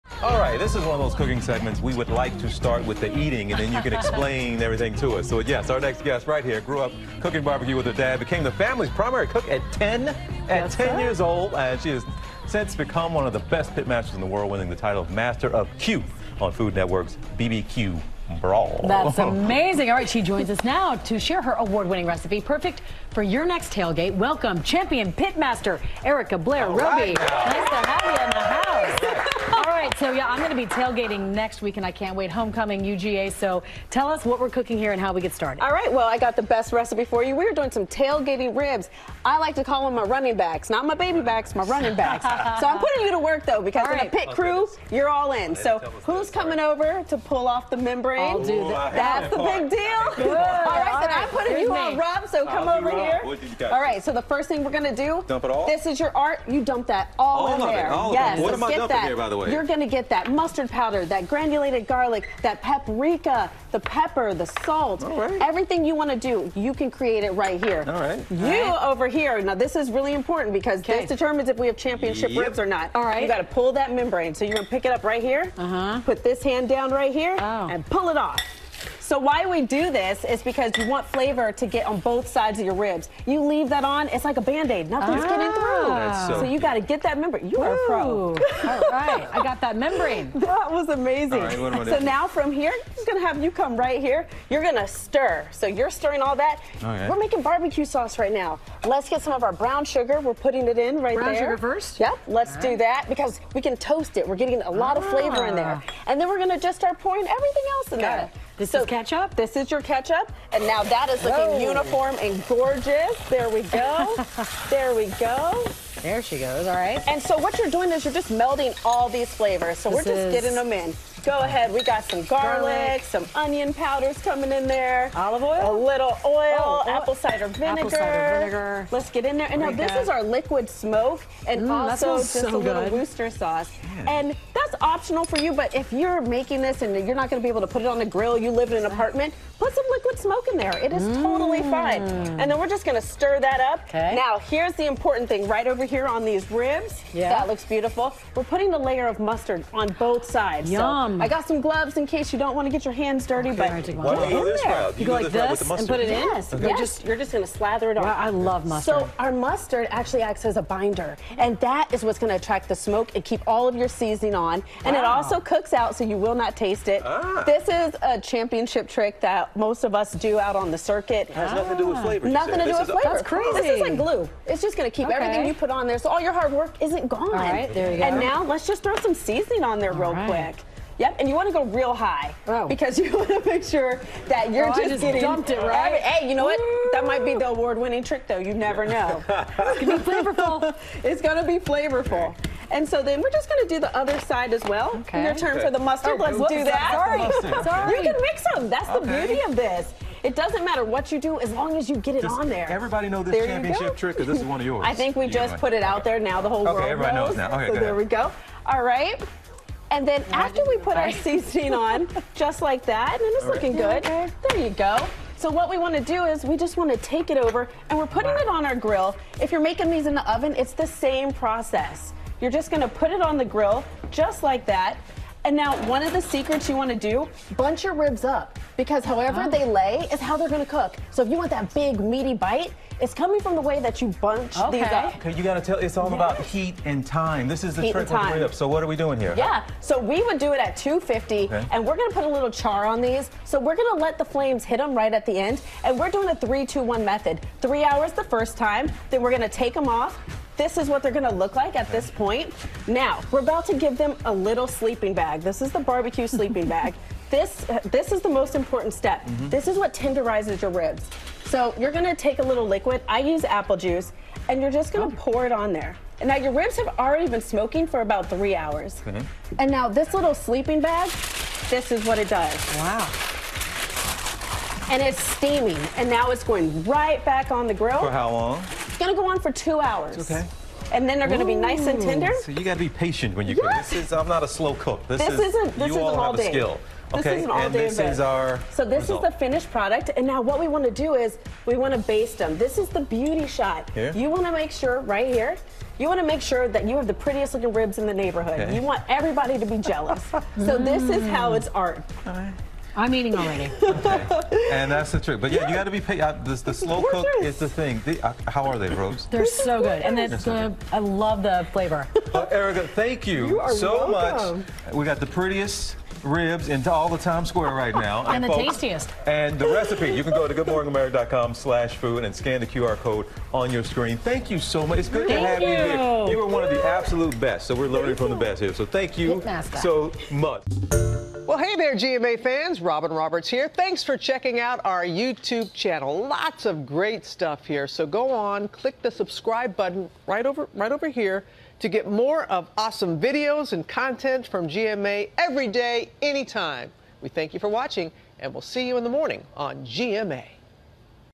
Pitmaster Erica Blaire Roby shares tailgating recipe
Champion Pitmaster Erica Blaire Roby shares BBQ tips and an award-winning ribs recipe.